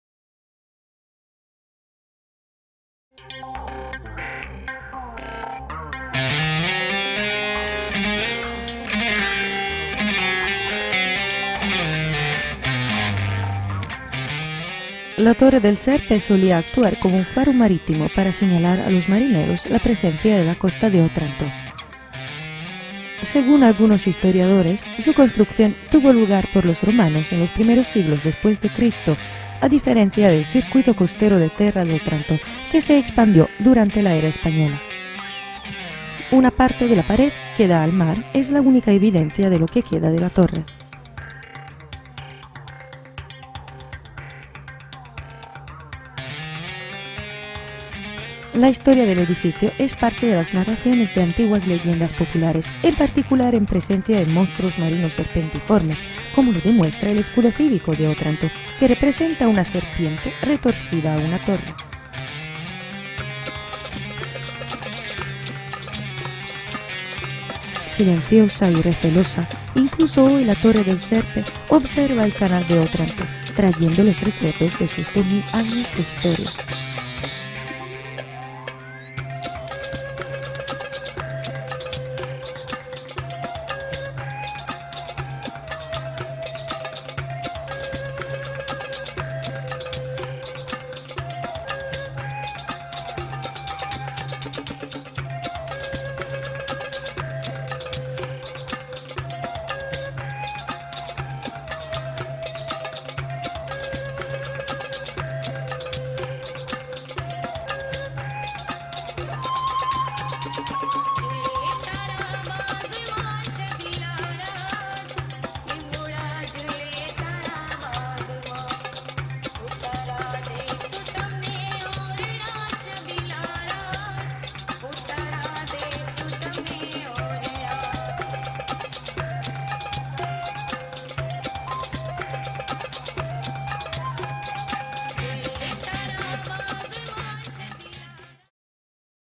Ciudad de Otranto - Torre del Serpe
La Torre del Serpe solía actuar como un faro marítimo para señalar a los marineros la presencia de la costa de Otranto.
Según algunos historiadores, su construcción tuvo lugar por los romanos en los primeros siglos después de Cristo, a diferencia del circuito costero de Terra d'Otranto que se expandió durante la era española.